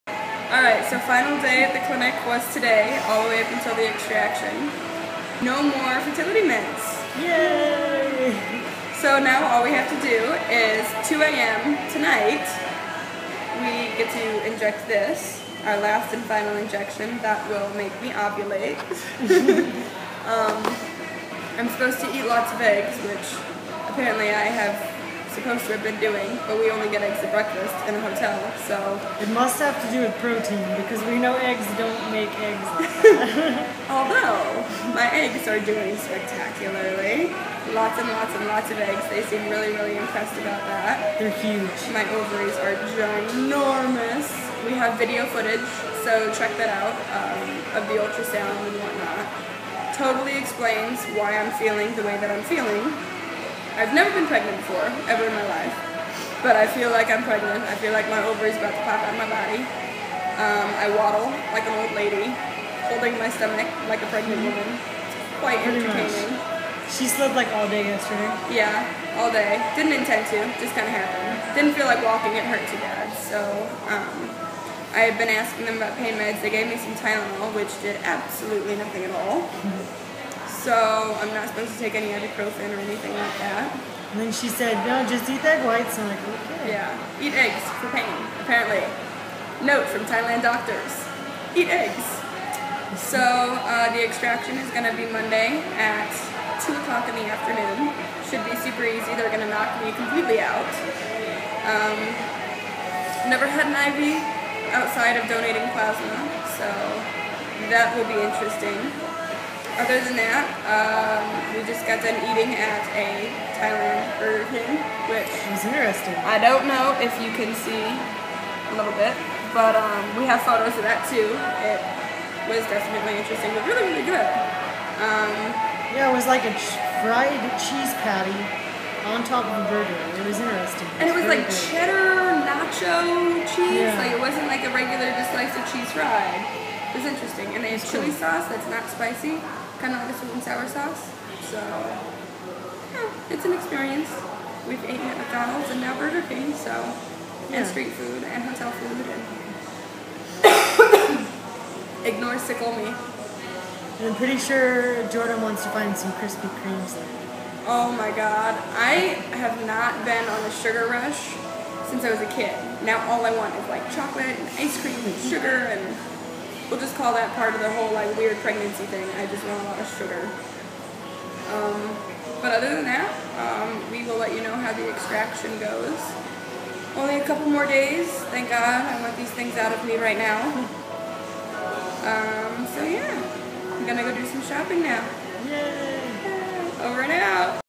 Last check up at the IVF clinic before retrieval
So my body at this point is going crazy with all the hormones and not being able to ovulate. So LOTS of pregnancy side effects, which i'm told is normal.  Quite an interesting experience.